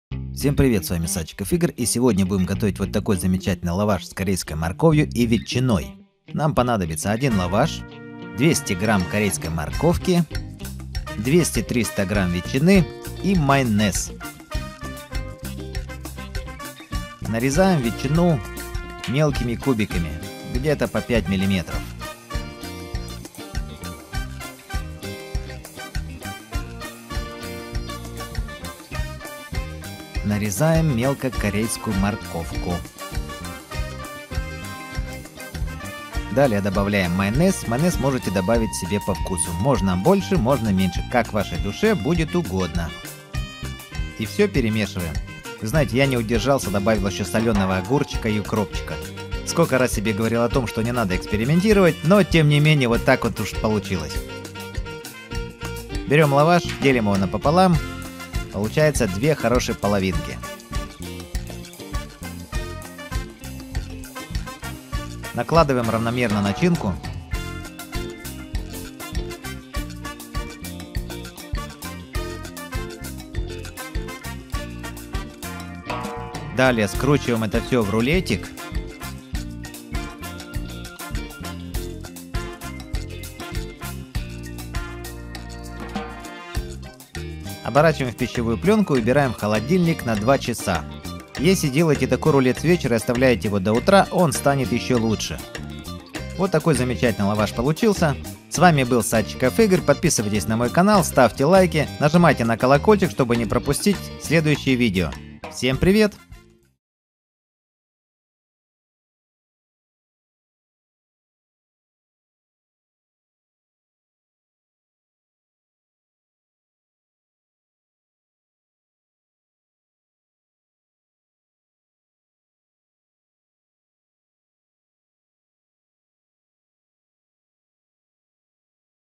Быстро и вкусно | Лаваш с корейской морковкой и ветчиной
Быстро и вкусно, как приготовить лаваш с корейской морковкой и ветчиной. Вкусный лаваш с начинкой.